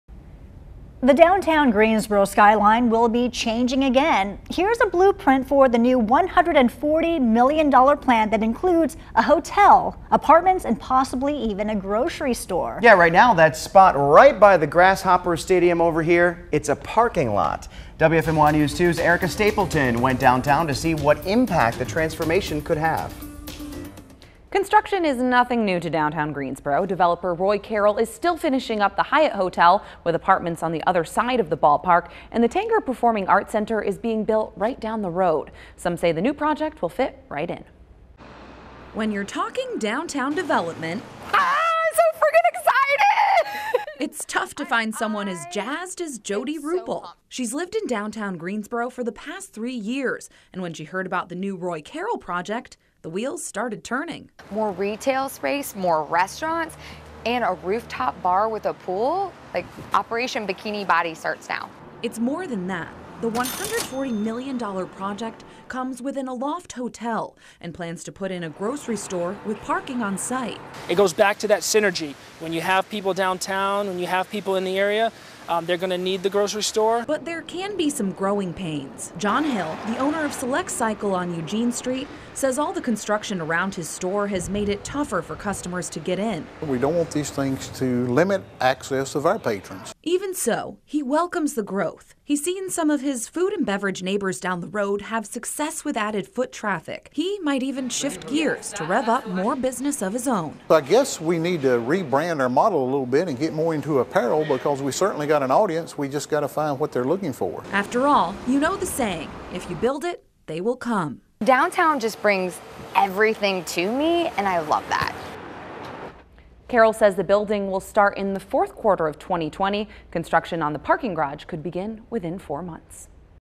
New Hotel Plan Downtown Greensboro
Developer Roy Carroll unveiled a plan for a new hotel, causing a lot of excitement in downtown Greensboro.